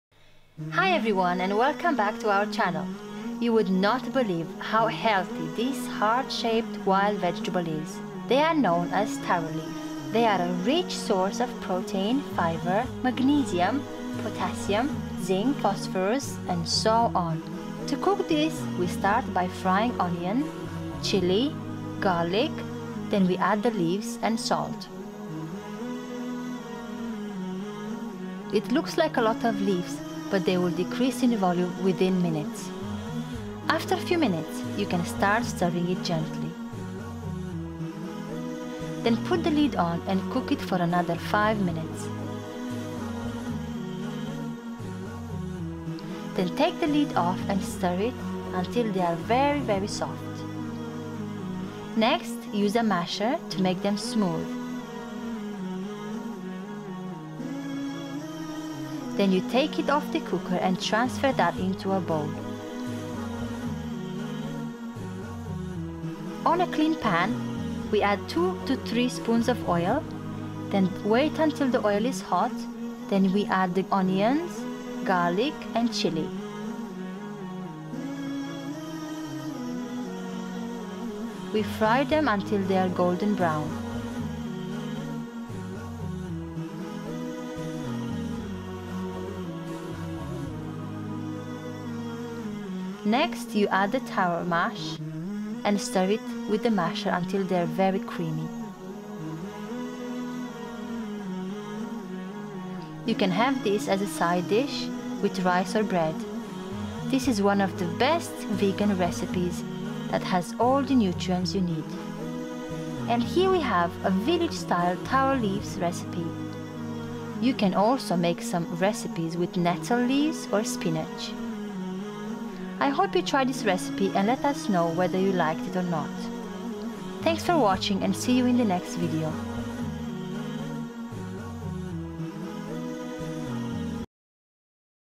Taro Leaf Recipe | Healthy Vegan
Taro leaf recipe is something we do all the time.
You would not believe how healthy this heart-shaped wild vegetables, they are known as taro leaf. They are a rich source of protein, fibre, magnesium, potassium, zinc, phosphorus and so on.

To cook this we start by frying
Ingredients:
100ml oil
2 onion
6 chilli
4 cloves of garlic
1kg taro leaves
and salt to taste

We love this village style taro leaves [ কোচুশাক ] recipe. You can have this as a side dish with rice or bread. This is one of the best veganrecipes that have all the nutrients you need. You can also make the same recipe with nettle leave or spinach. I hope you like this villagecooking and please let us know if you did in the comments.